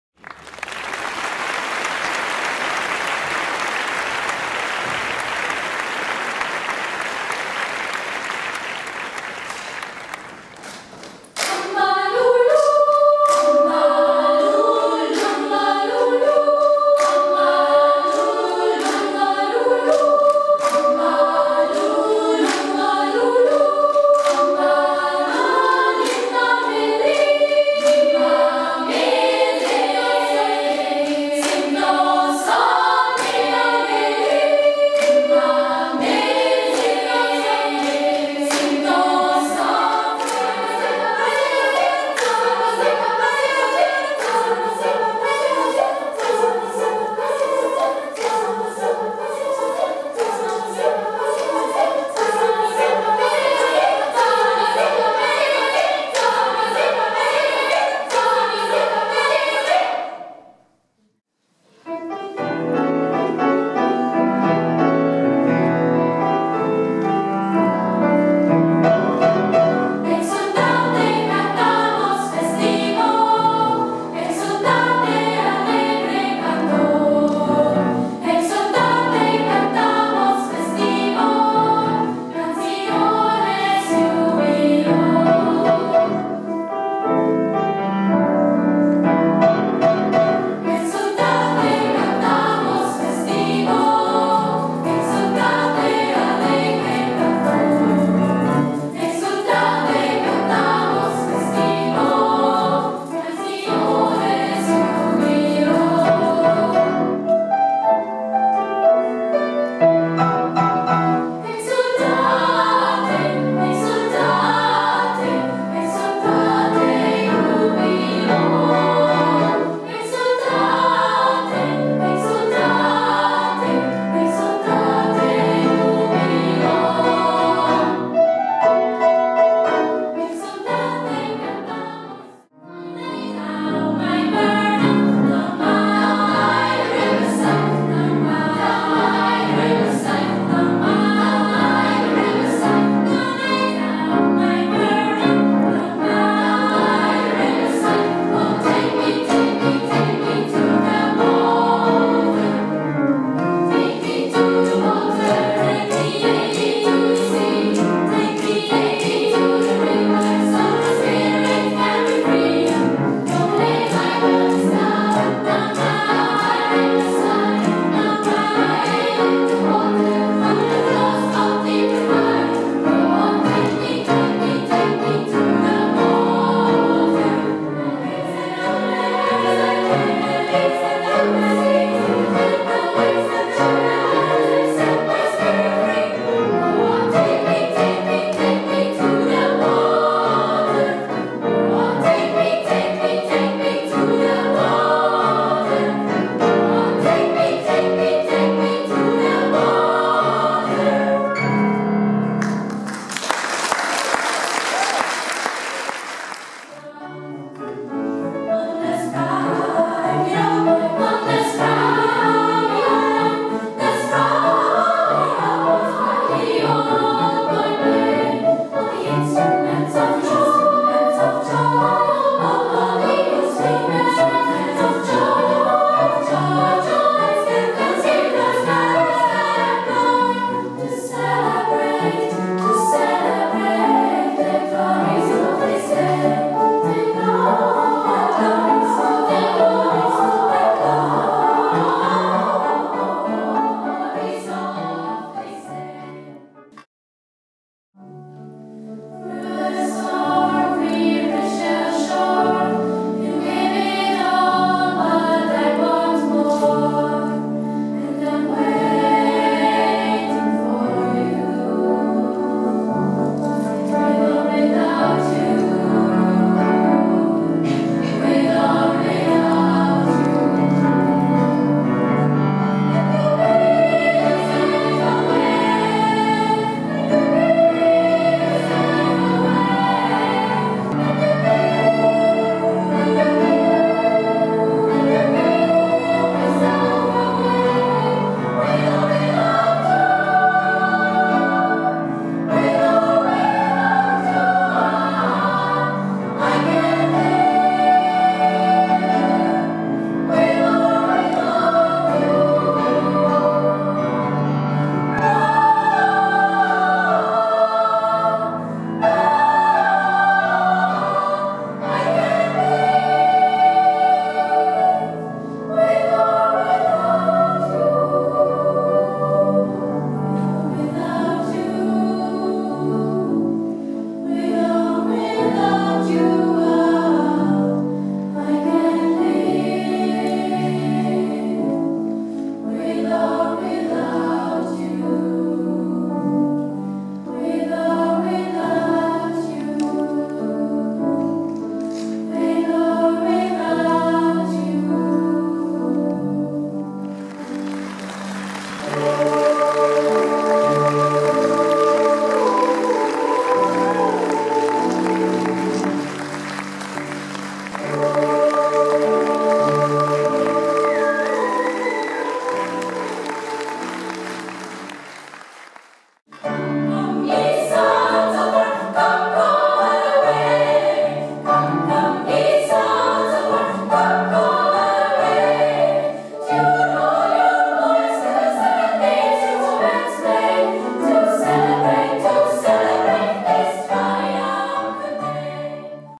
Stimmwerk 2015
Ein Rückblick auf die Projekte vom Frühling/Sommer 2015 mit dem Favoritchor des Solothurner Mädchenchors.

Johanna Scherer, Klavier
Lea Scherer, Leitung